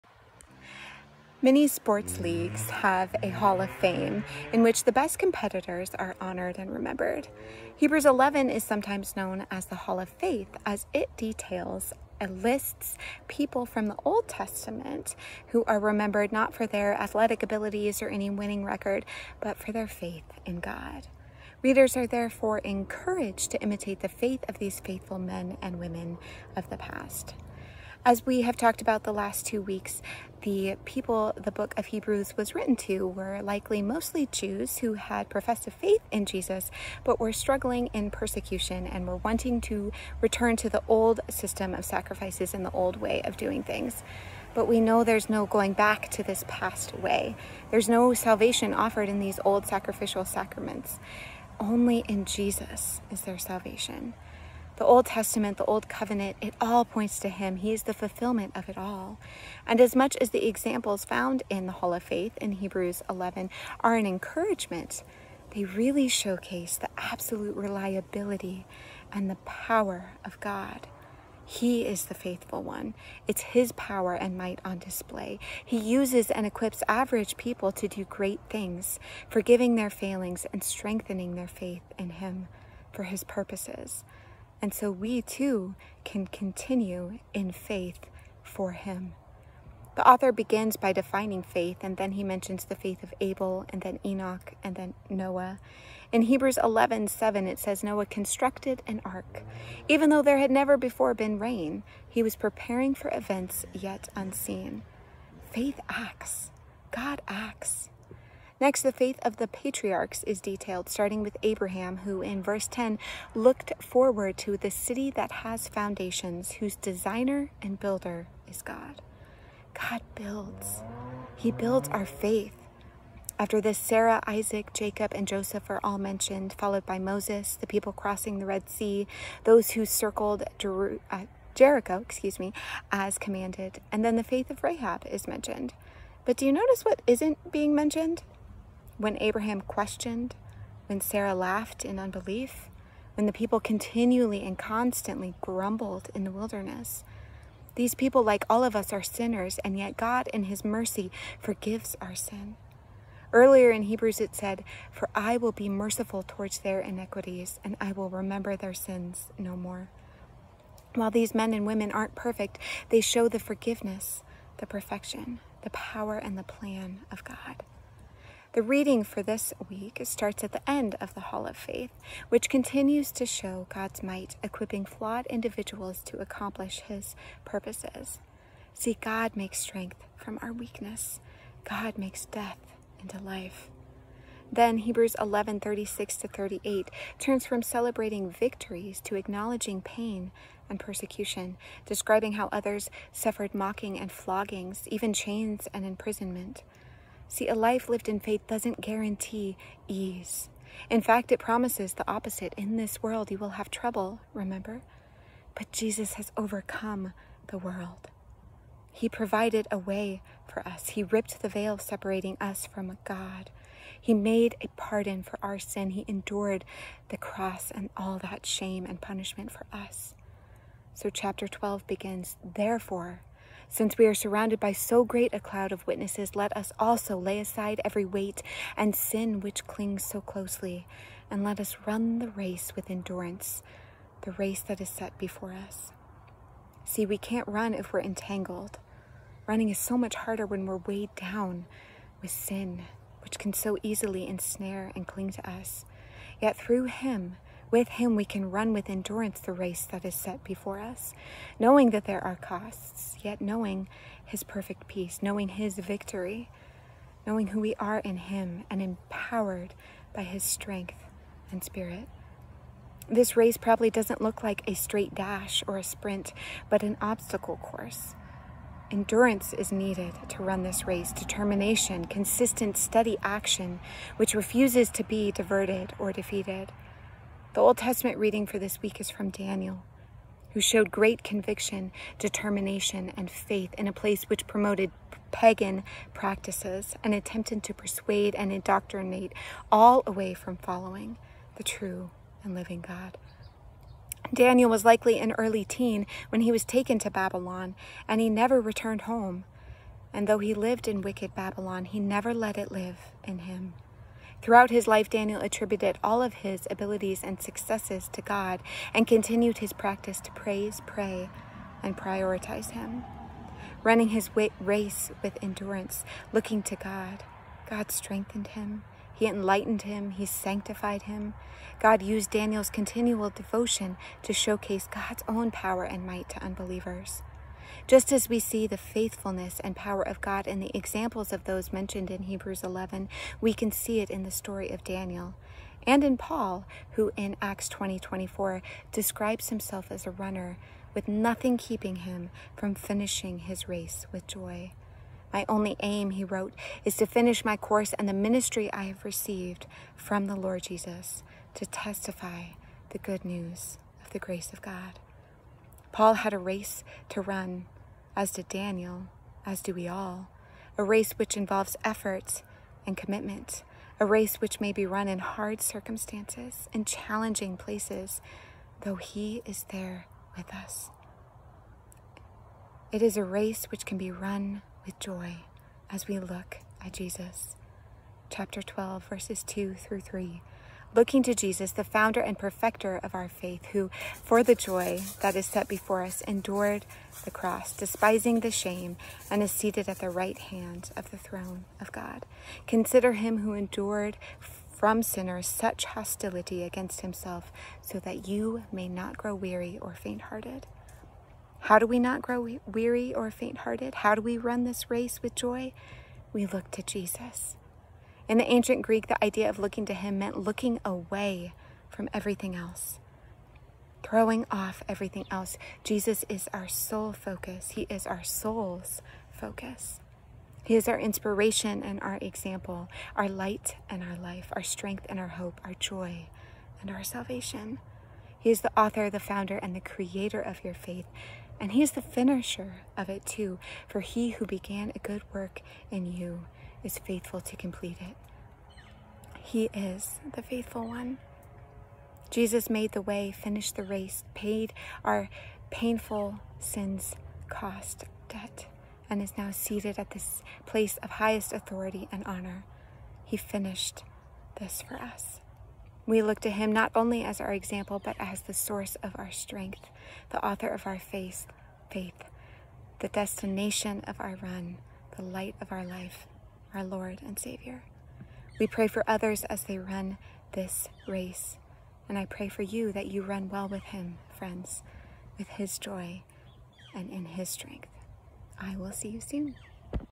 Bible Study for October 15th, 2023
The Bigger Story, Week 42: Bible Study on Hebrews 11 and 12.
Look to Jesus, the founder and perfecter of our faith and keep Him your sole (soul's) focus!
Join us in-person for our Morning Bible Study at 8:30 am each Sunday!